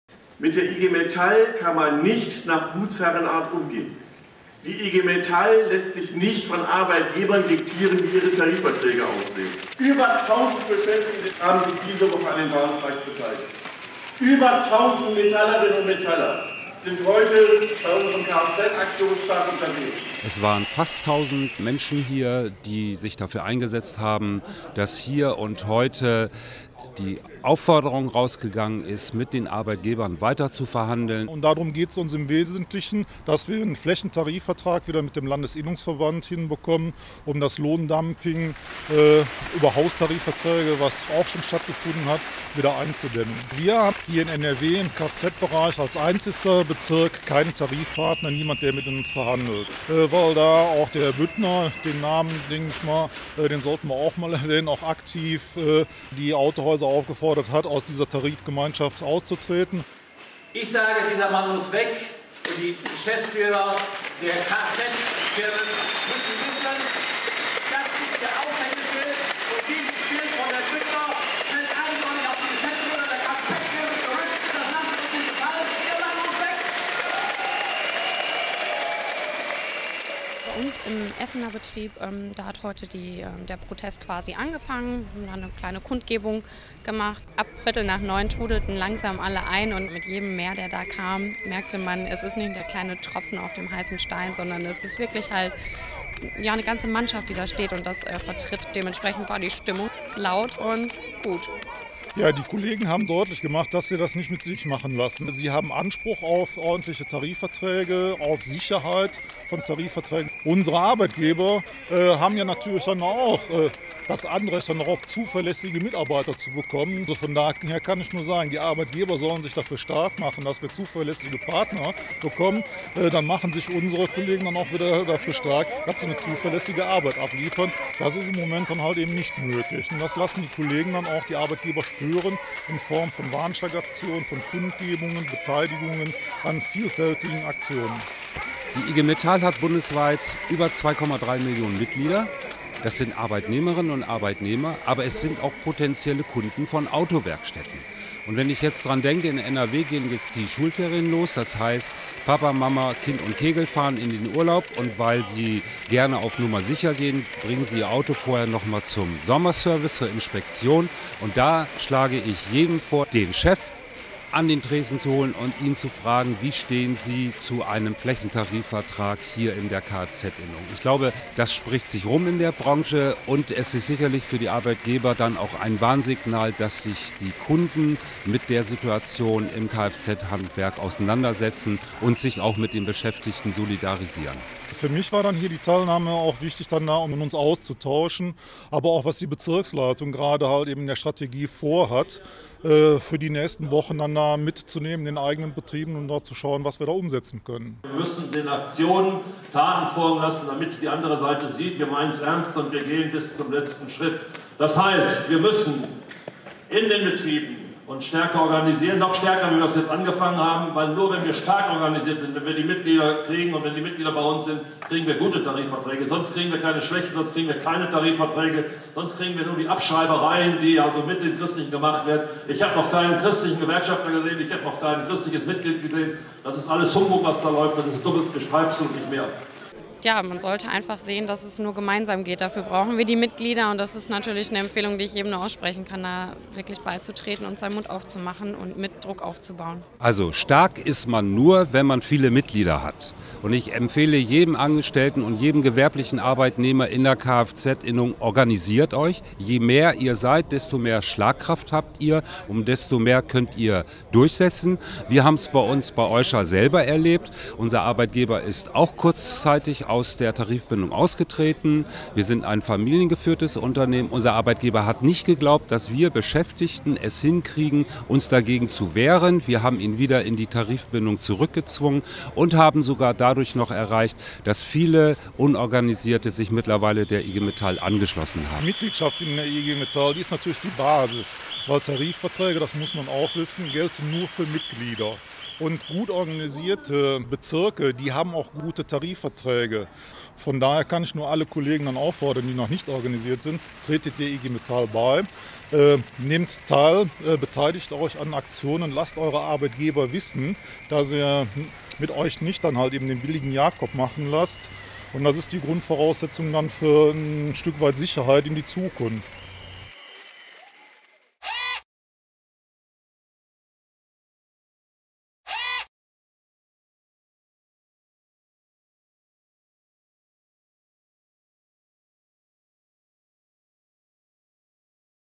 Kfz-Aktionskonferenz mit vielen Ideen
650 Kfz-Handwerker, Betriebsräte und IG Metall-Sekretäre haben sich in der Messe Essen getroffen, um den Druck auf die Autohäuser und Kfz-Innungen zu steigern. Knapp 85.000 Beschäftigte in NRW warten auf eine Lohnerhöhung.